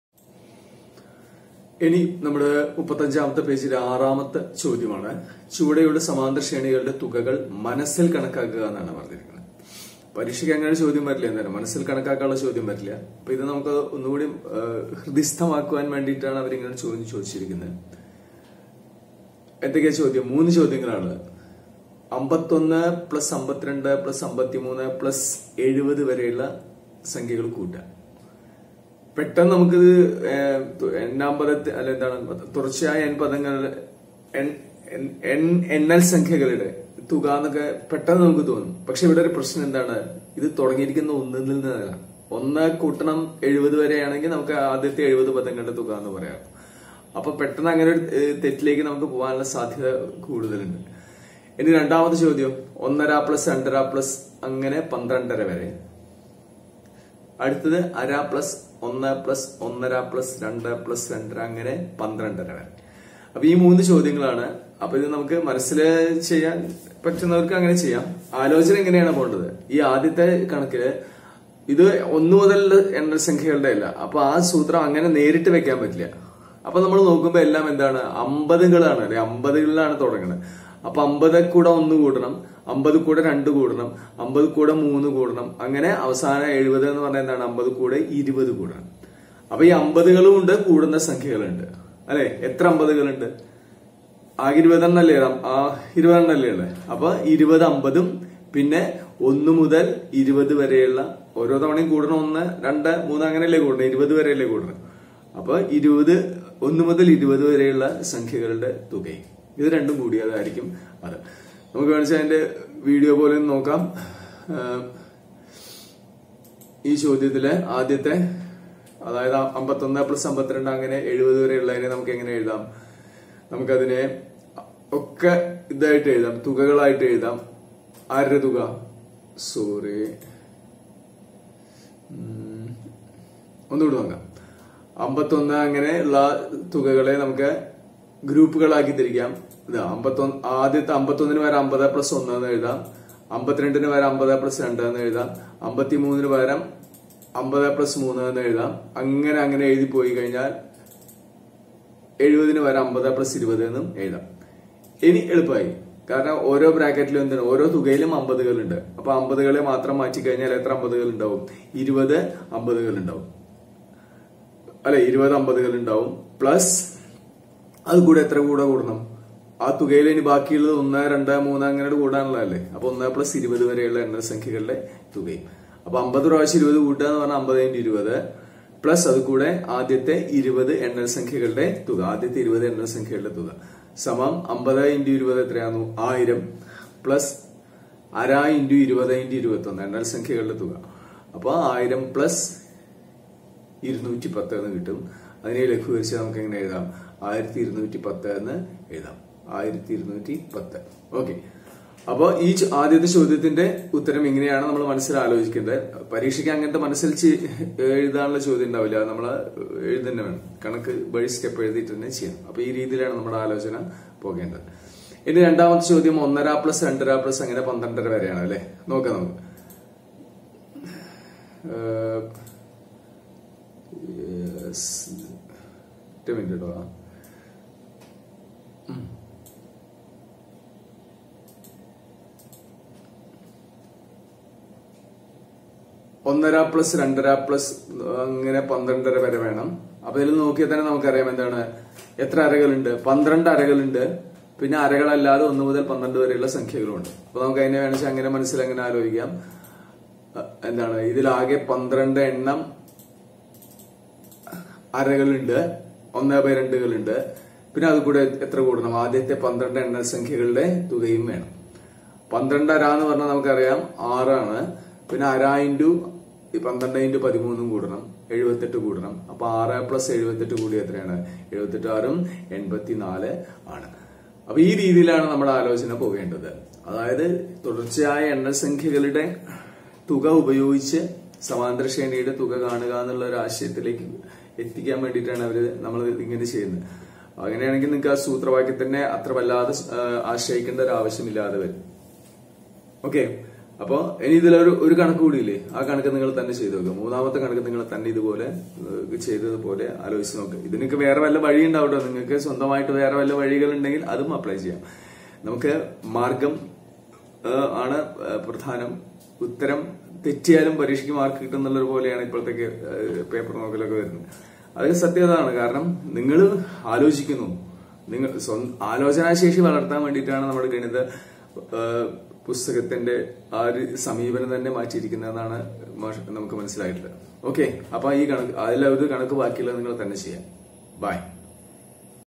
Online Maths Class : 12
Page 35 ലെ ആറാമത്തെ ചോദ്യത്തിൻ്റെ വിശദീകരണം